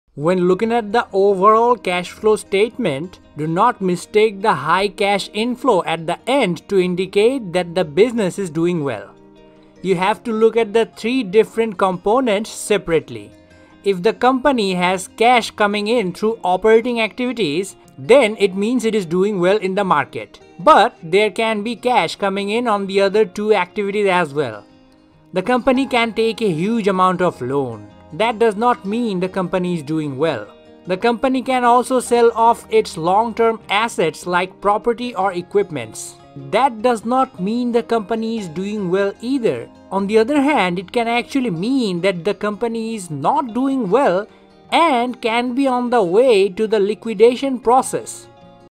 Cash Flow Statement and Company Health
Sometimes the overall high cash flow might make it seem like that a business is doing really well. But you have to look closely at the cash flow statement and the cash flow from operating activities.
Because sometimes, high cash flow can come from the company selling aways most of its assests, or taking a huge amount of loan.
When looking at the overall cash flow statement, don’t mistake the high cash inflow at the end to indicate that the business is doing well.
You have to look at the three different components separately. If the company has cash coming in through operating activities, then it means it is doing well in the market.
But, there can be cash coming in on the other two activities as well. The company can take a huge amount of loan. That doesn’t mean the company is doing well.  where the company can sell off its long term assets. That clearly means the company is not doing well and can be on the liquidation process.